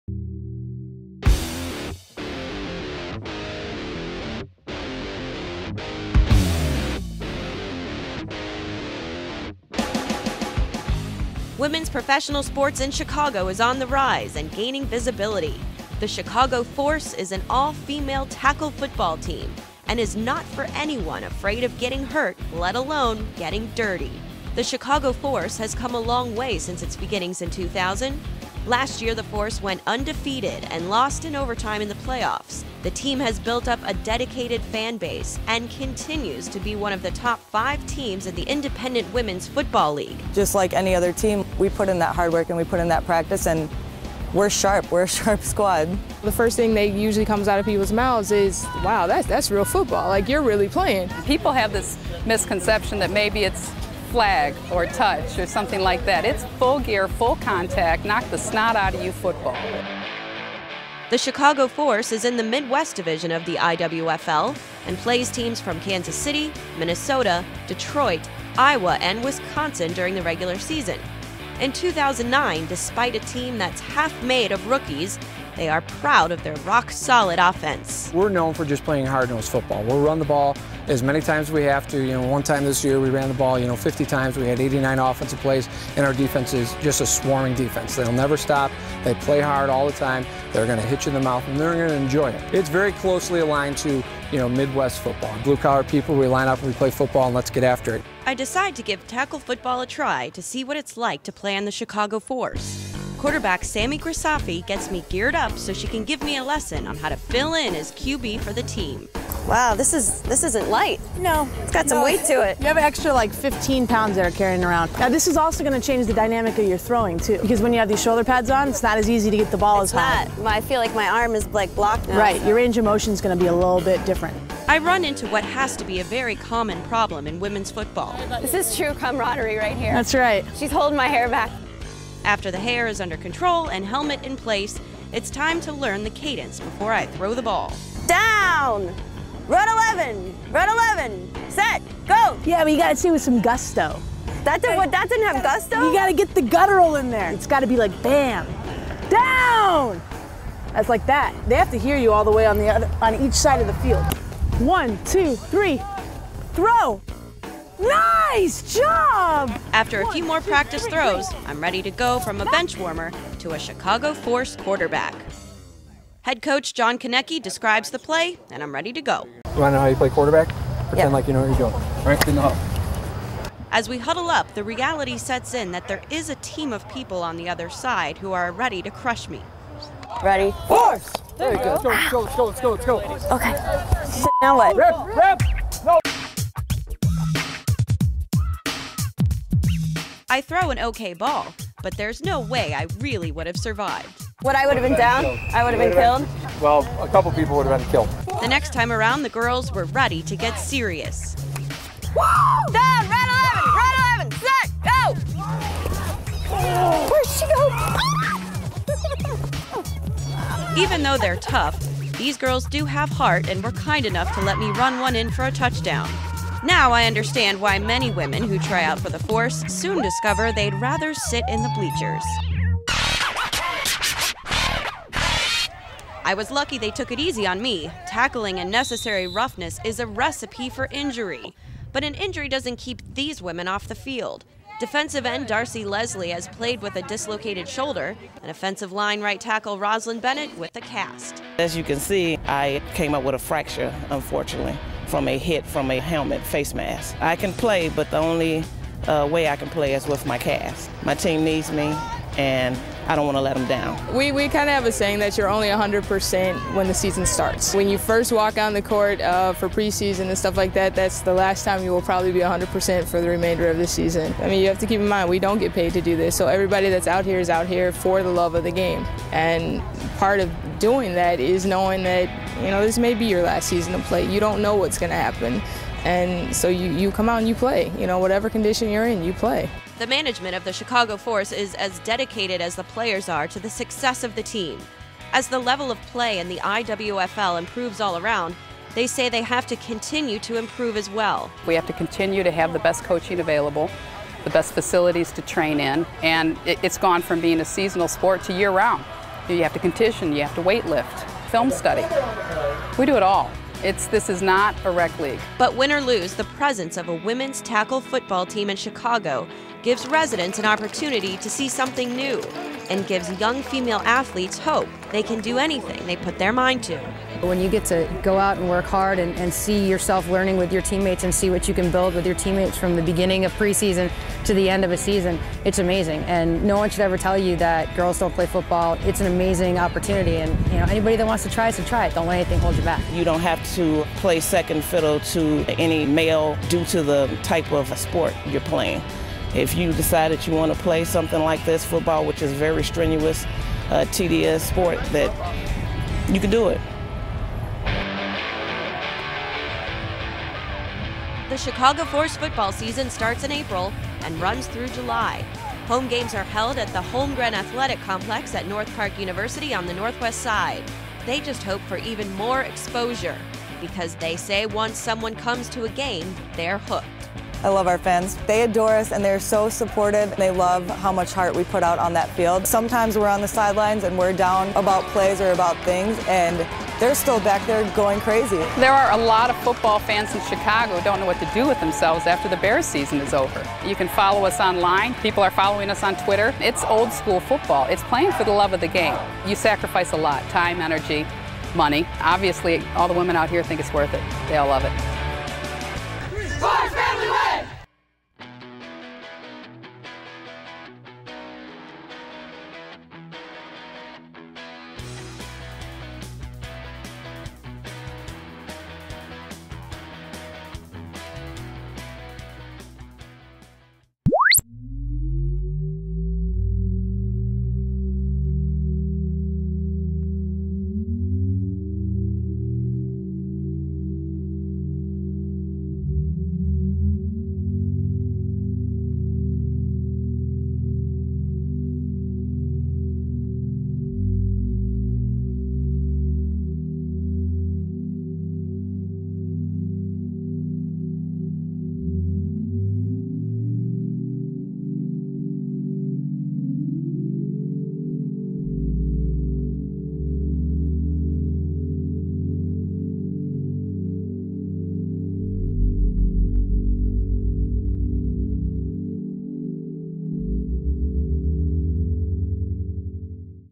Chicago Force Part 2
The ladies on the Chicago Force play football just like the guys do.  We get an inside look at how they play, and host Katie Kijowski gears up to play with the team.